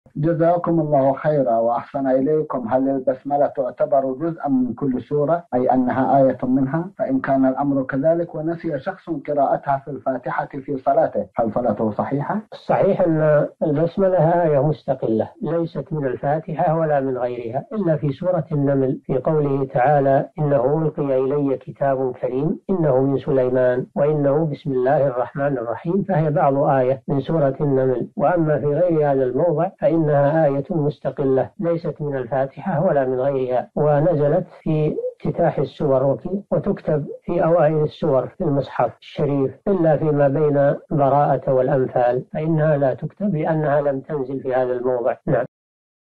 هل البسملة تعتبر جزءا من كل سورة وإذا كان كذلك ونسي شخص قراءتها في الفاتحة في صلاته، هل صلاته صحيحة؟
نص السؤال: هل البسملة تعتبر جزءًا من كل سورة، أي أنها آية منها، فإن كان الأمر كذلك ونسي شخص قراءتها في الفاتحة في صلاته، هل صلاته صحيحة؟
السيرة الذاتية :
للاشتراك في قناة الشيخ صالح الفوزان

رئيس الفريق
لمشروع كبار العلماء